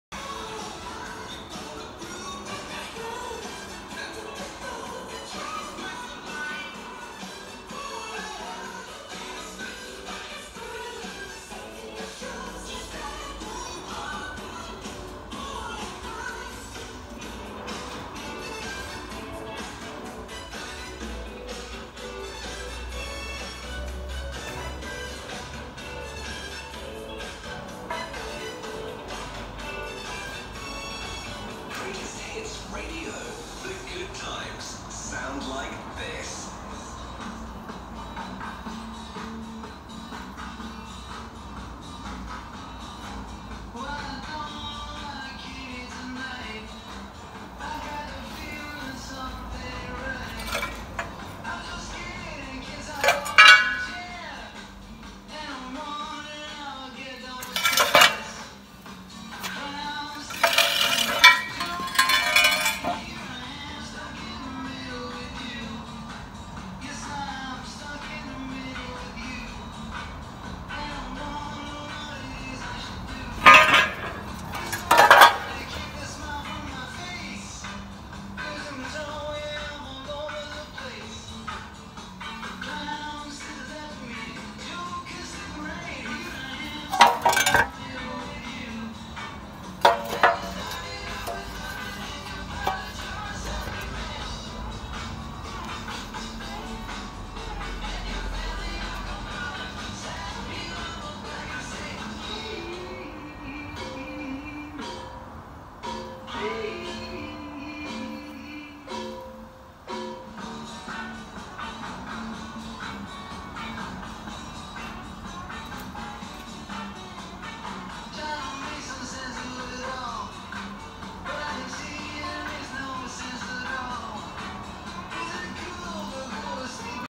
Bentley Mk6 chassis modification
This is a chassis from a Bentley Mk6 that I modified to move the engine back